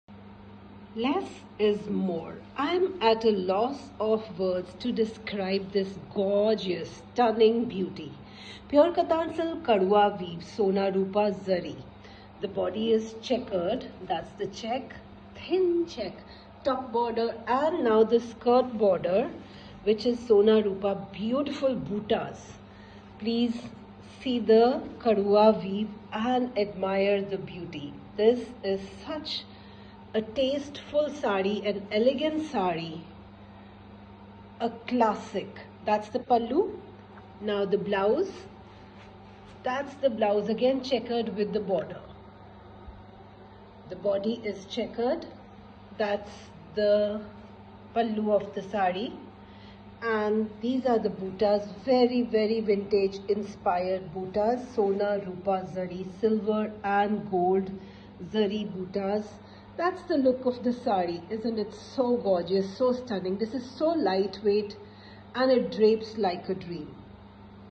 pure katan silk saree
Immerse in the timeless allure of cream, gold, and silver hues, woven delicately in pure katan silk through intricate kaduwa weave. Vintage-inspired butas and checks adorn the fabric, telling tales of tradition and craftsmanship. A symphony of history and artistry, captured in every thread.

"Discover the magic of handmade sarees! 🌟 Embrace elegance and tradition with our exquisite collection. HandmadeSarees EthicalFashion SustainableStyle Happy shopping!